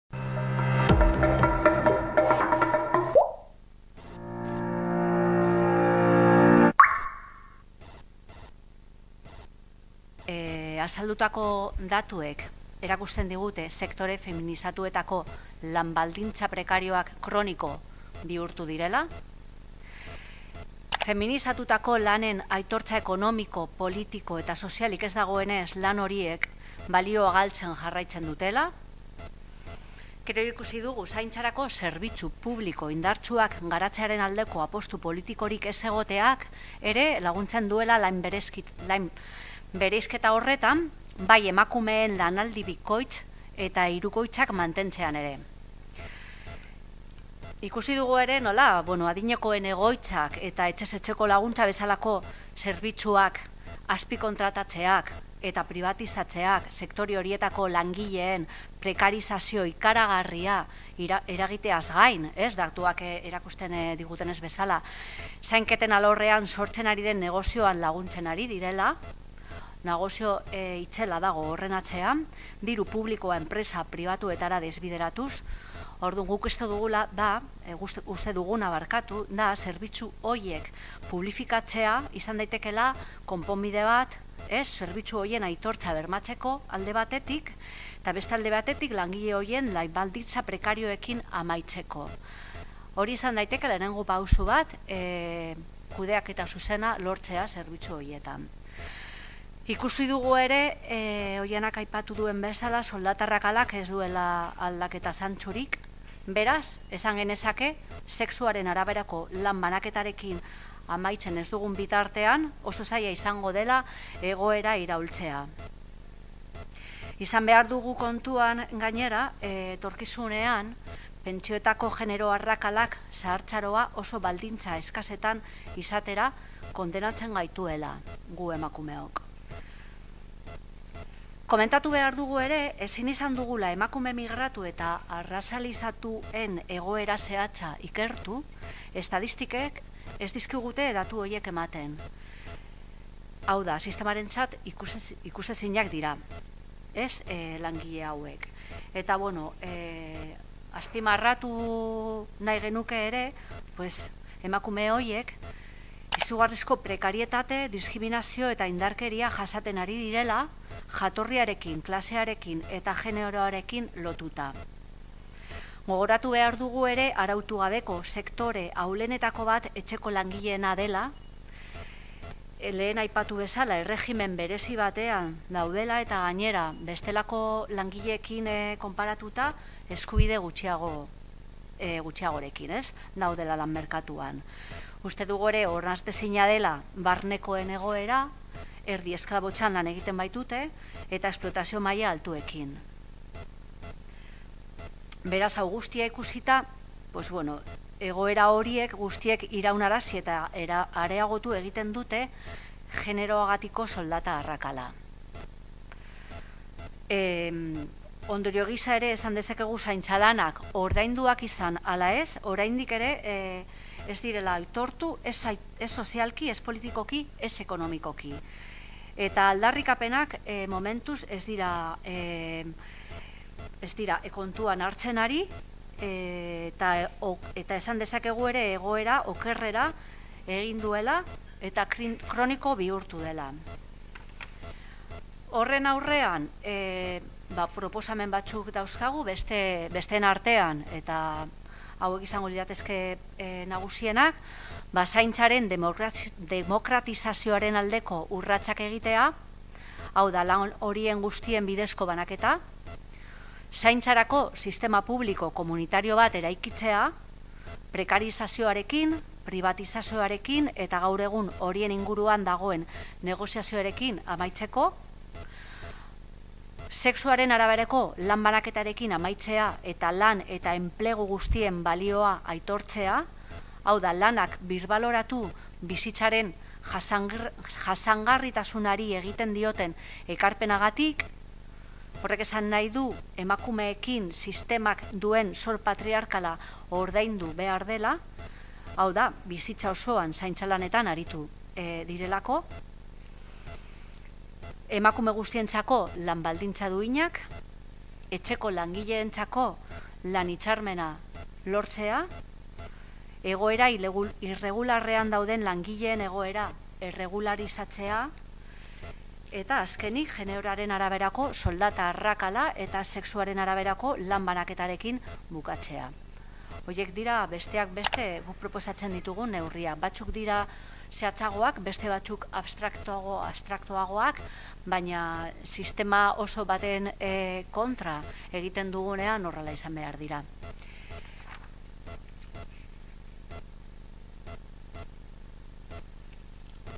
"Sektore feminizatuen egoera orokorra Hego Euskal Herriko enpleguan" txostena aurkeztu dugu
Lanaren sexu-banaketarekin eta berak dakarrenarekin amaitzen ez dugun bitartean, oso zaila izango da soldata-arrakalarekin bukatzea